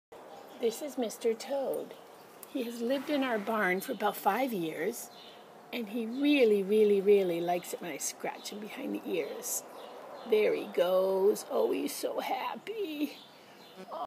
Edward toad
We've had a toad living in our barn for the last 5 years. He's not afraid of  horses dogs or human